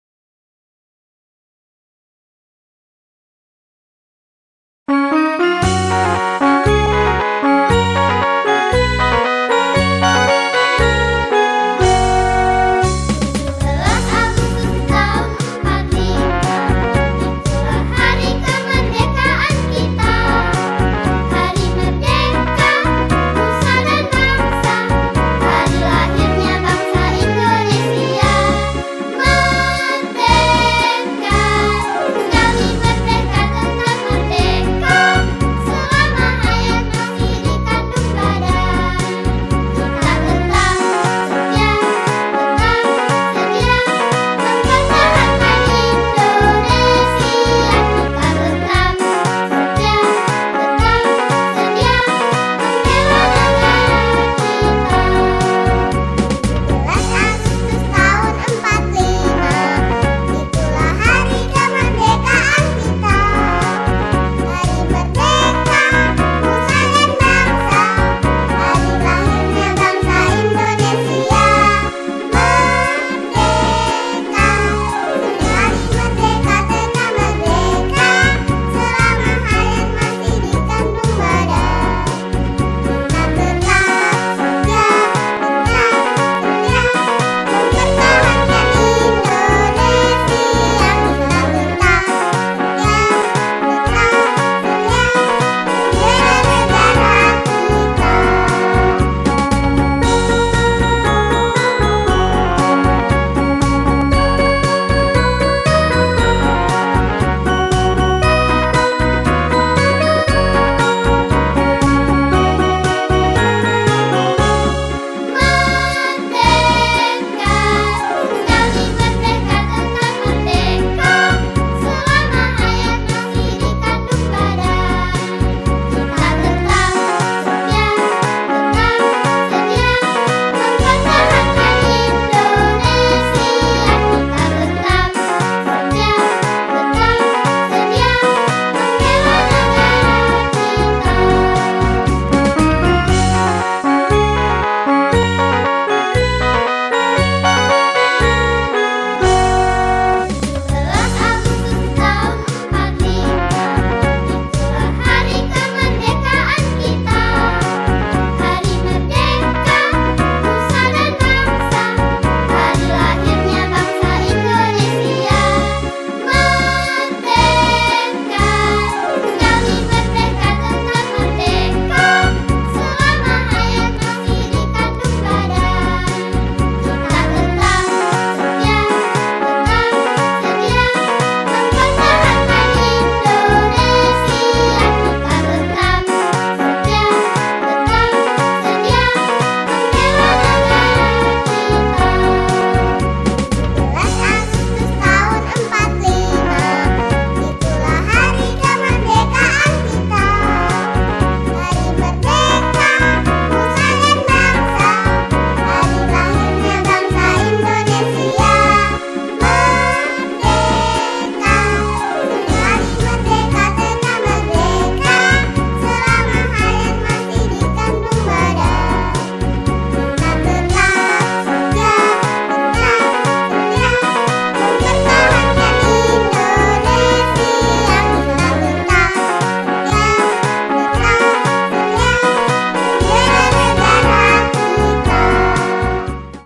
TAMADA FAMILY GOES TO MUSEUM
TK & KB TAHFIDZUL QUR'AN PERMATA BUNDA MELAKUKAN KUNJUNGAN KE MUSEUM RONGGO WARSITO SEMARANG